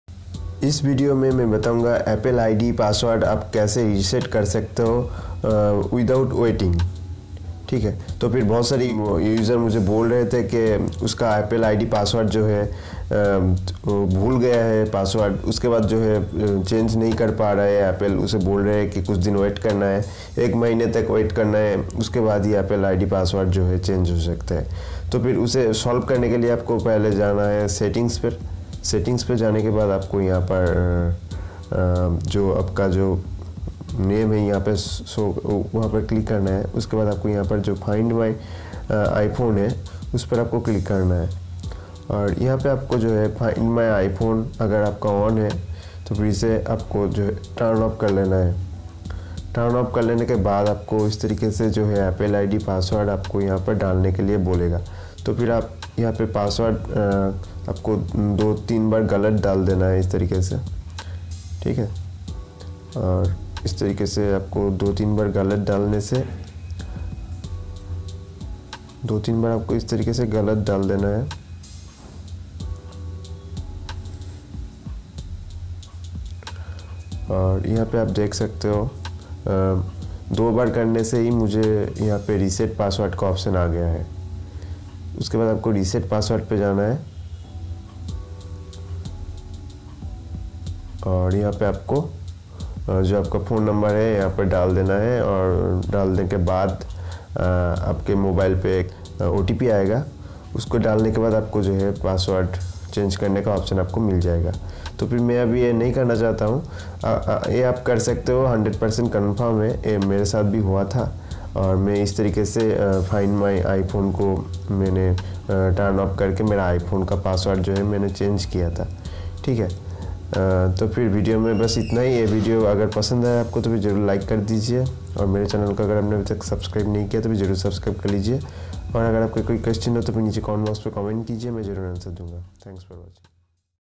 How To Reset Apple ID Password Without Waiting 2020 | Change Apple ID Password
How To Reset Apple ID Password Without Waiting 2020
Change Apple ID password